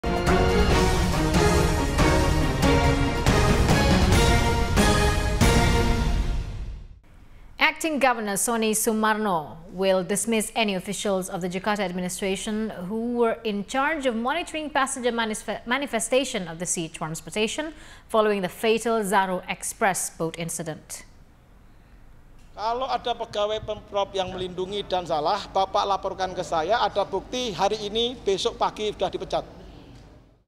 Captain Of Zahro Express Boat Named Suspect
Captain M. Nali is named suspect as he allegedly neglected maritime safety, as he allowed the boat to sail although he knew it was overcapacity.